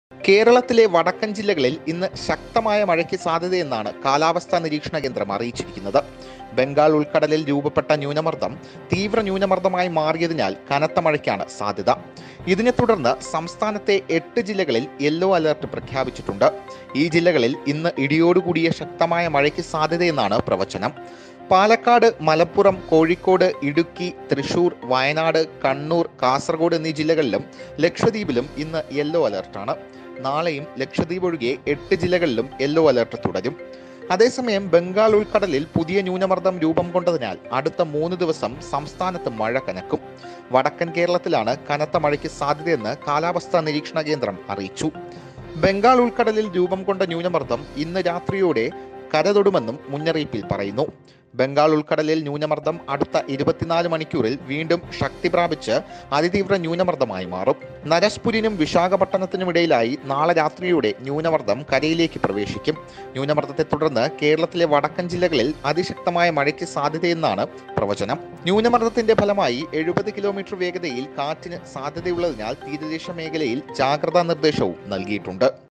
കേരള: തീവ്ര ന്യൂനമർദം; വടക്കൻ ജില്ലകളിൽ കനത്ത മഴയ്ക്ക് സാധ്യത; എട്ട് ജില്ലകളിൽ യെല്ലോ അലർട്ട്
Kerala: Severe hypotension; Chance of heavy rain in northern districts; Yellow alert in eight districts